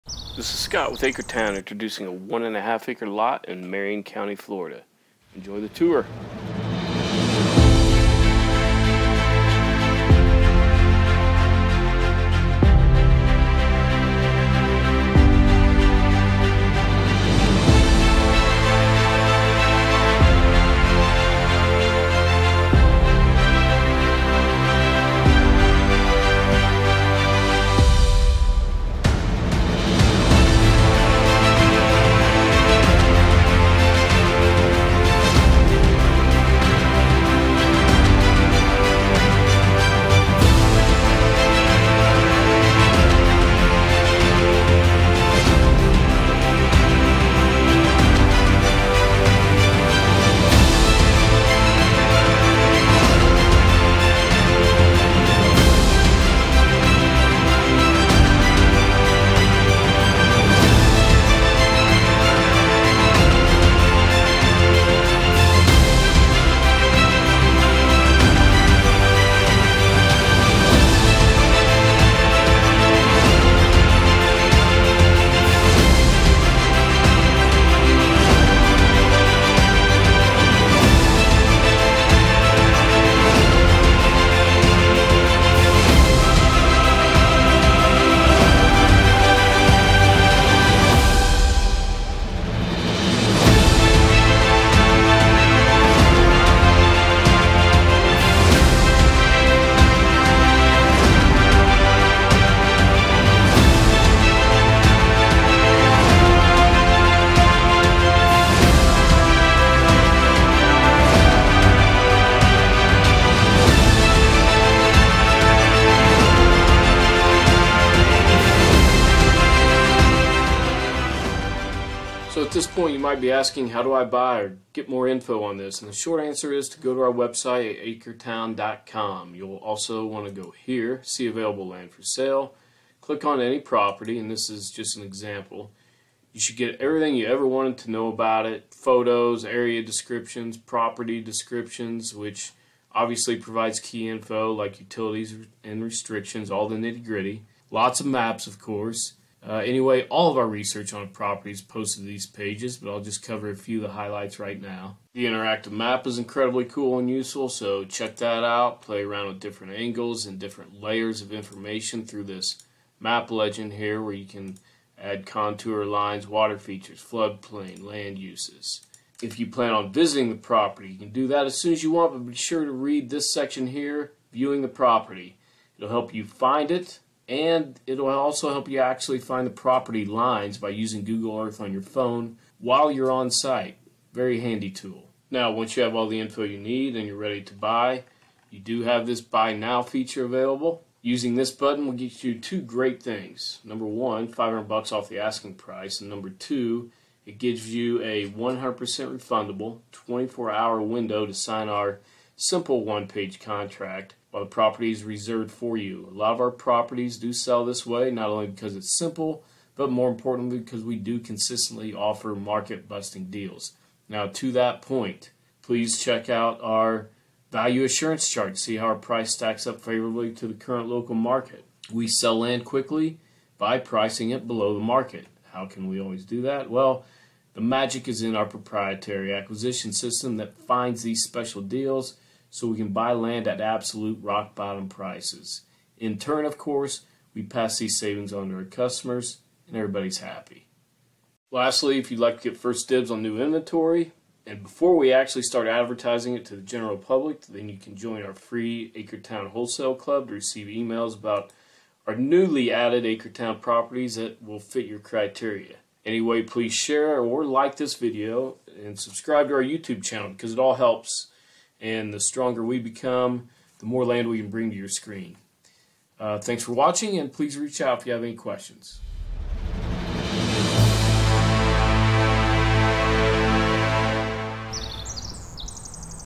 Owner Financed Land in Florida | 7 mins to Lake & Major Shopping | No Floodplain | Partly Cleared
Cheap Land in Florida near Lake Weir

Only 7 minutes from Eaton’s Beach on Lake Weir and 9 minutes from major shopping like Walmart and Lowe’s, this peaceful property is near everything you want.

- Public Road Frontage
- Only 7 mins to Lake; 9 mins to Major Shopping
- Surveyed
- Very Buildable: Partially Cleared, Flat, No Floodplain
- Power Available
- Easy Zoning (Mobile Homes OK)
- Priced Well Below Market

PROPERTY: This tranquil property boasts 1.5 acres of potential. Nestled in a peaceful, sparsely developed neighborhood, this parcel is the perfect spot to build your dream home. With no floodplain encroachments, and partially cleared and flat terrain, you can rest easy knowing that you’ll be building in no time. Grade your driveway anywhere within the 119 feet of public road frontage on SE 176th, which is lined with power. Its A-1 Agricultural zoning allows a wide spectrum of uses. From mobile homes to horse ranches to chicken farms and more, design your best life here, and let the available survey guide your way as you build it. Location is everything, and this spot truly has it all. Just a short 7 minute drive away is Eaton’s Beach at Lake Weir, where you can spend sunny afternoons basking in the beauty of nature or casting a line. Need to run errands or indulge in a little retail therapy? No worries! Walmart, Target, Lowe’s, and a host of other major retailers are just 9 minutes away in Lady Lake, making shopping a breeze. Given such a serenely rural feel to your home base, you won’t believe how close you are to poppin’ urban action. Jump on this prime residential property today at an unbeatable Acretown price!

The best and most updated source of information on this property will always be